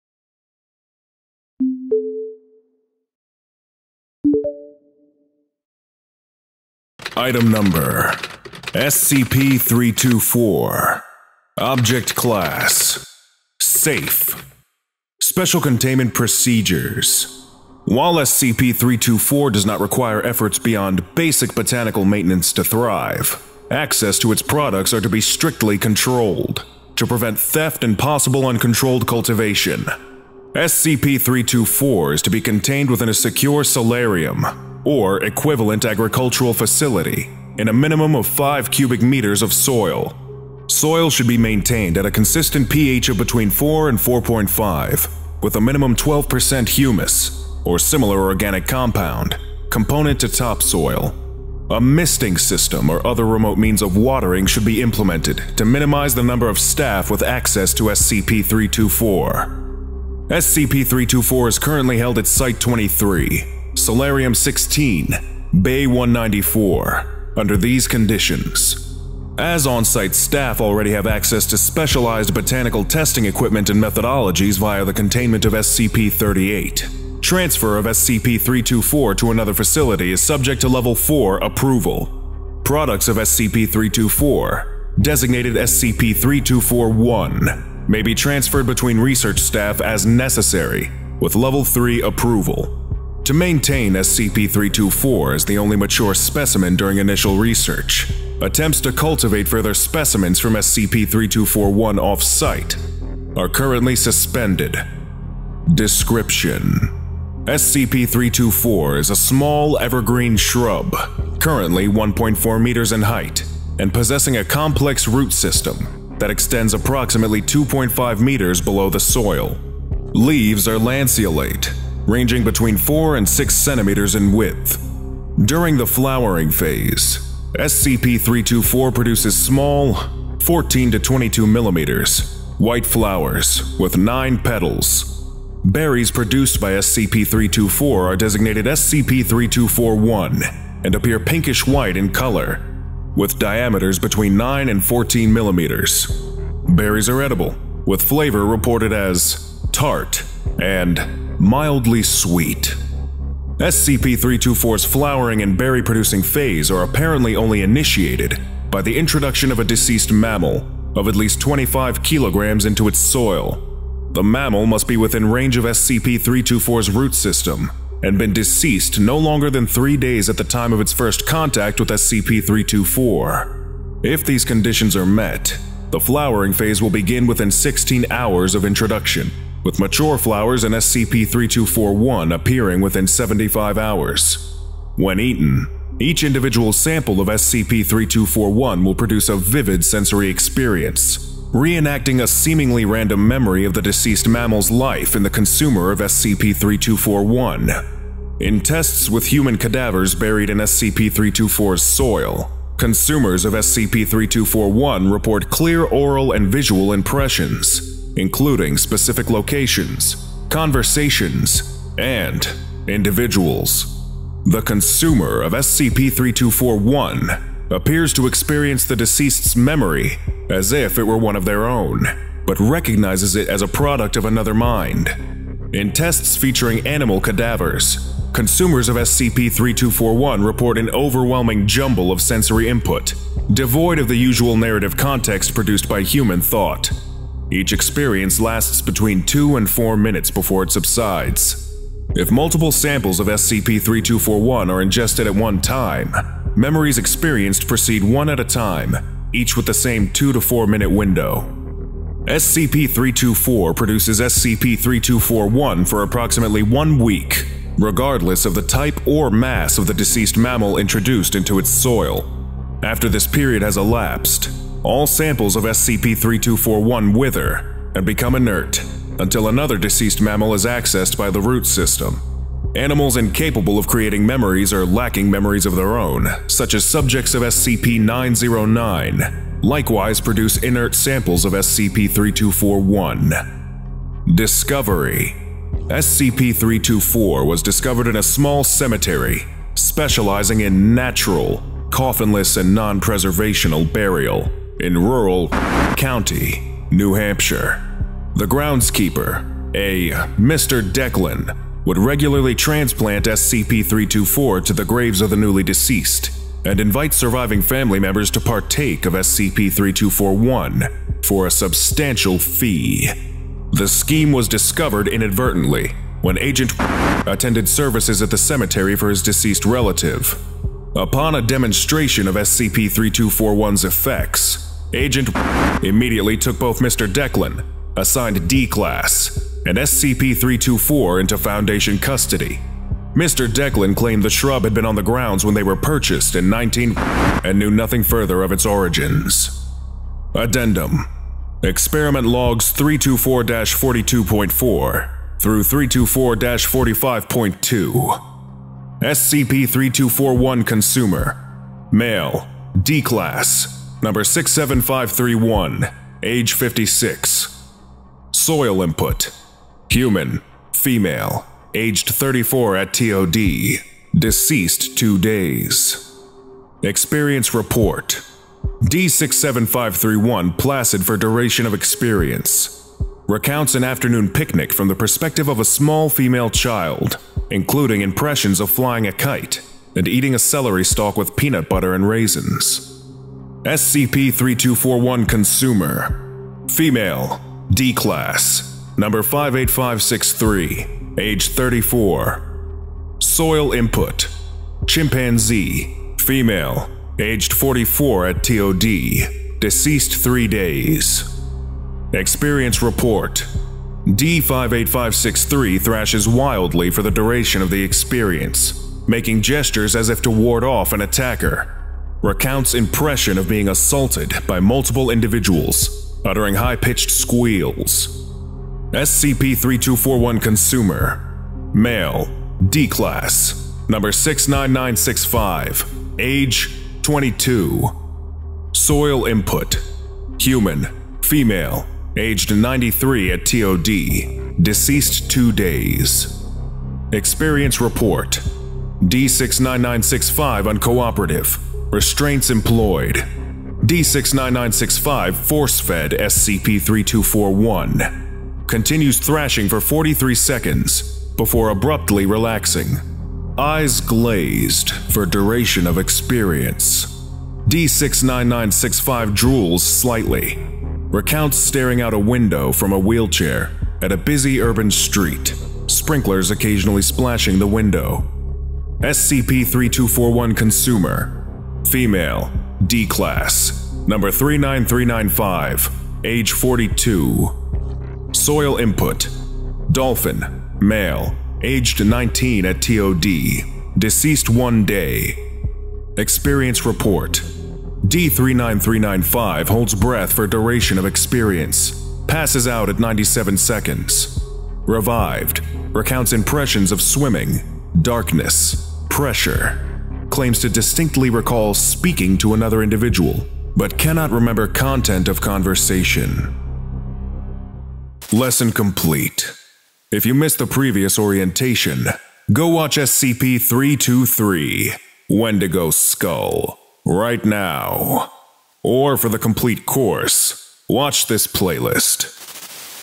SCP-324 | Eulogy Shrub (SCP Orientation)
Today we will be studying Item number SCP-324: Eulogy Shrub, Object class: Safe.

SCP-324 is a small, evergreen shrub currently 1.4 meters in height and possessing a complex root system that extends approximately 2.5m below the soil. During the flowering phase, SCP-324 produces small white flowers with nine petals. Berries produced by SCP-324 are designated SCP-324-1. Berries are edible, with flavor reported as tart and mildly sweet.

Any mammalian cadaver placed near the roots of SCP-324 is turned into SCP-324-1. When eaten, each individual sample of SCP-324-1 will produce a vivid sensory experience re-enacting a seemingly random memory of the deceased mammal's life in the consumer of SCP-324-1.

In tests featuring animal cadavers, consumers of SCP-324-1 report an overwhelming jumble of sensory input devoid of the usual narrative context produced by human thought.

and released under Creative Commons Sharealike 3.0. Contributor: Mulciber

Author: Leonora (Ellie) Enking from East Preston, United Kingdom
Author: unknown